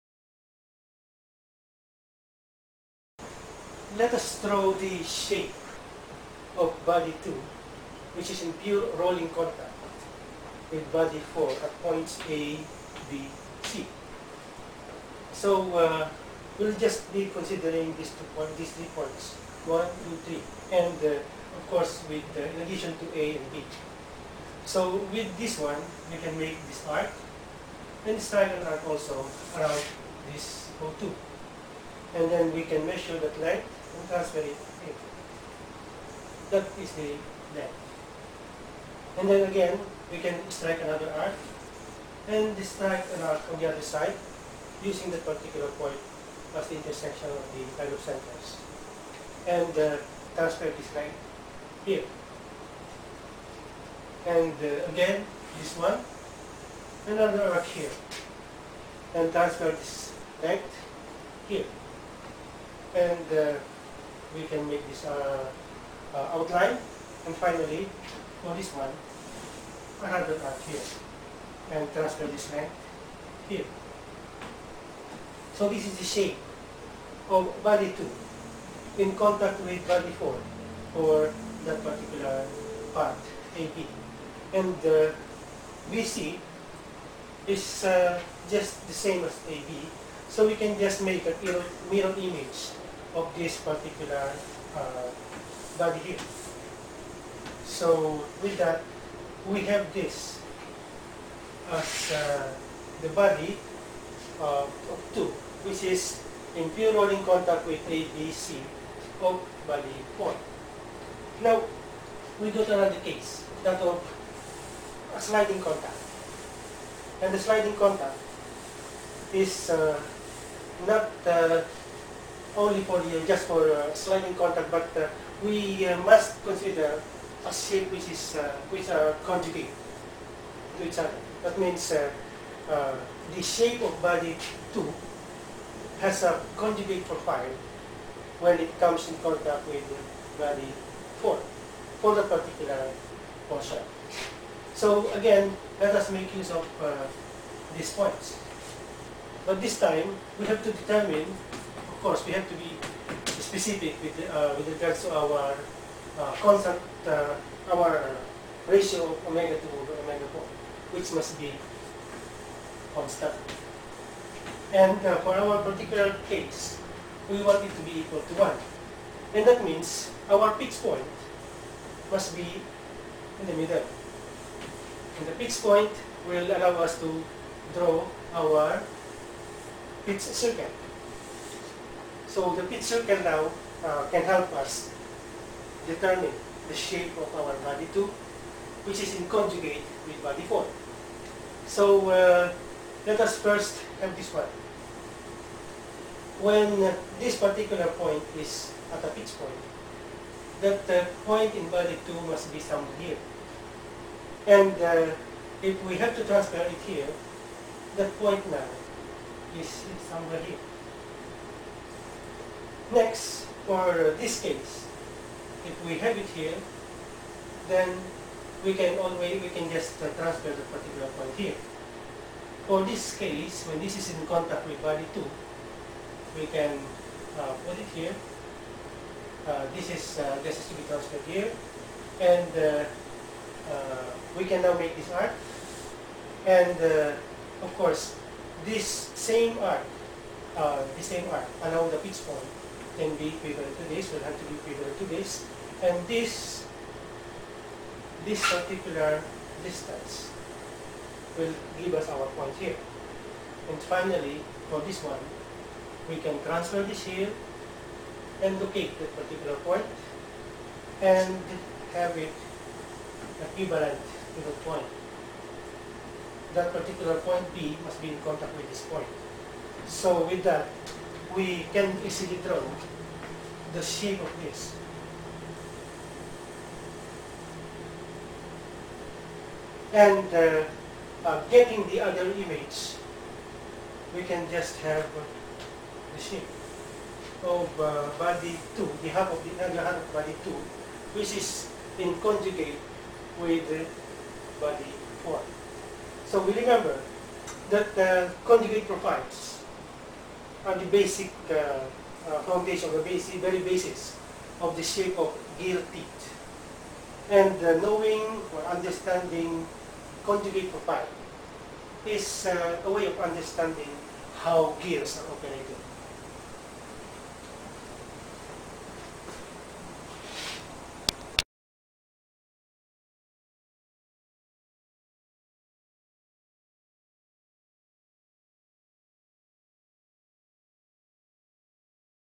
Rolling Contact and Sliding Contact
Principles of transmission of motion of direct contact mechanisms.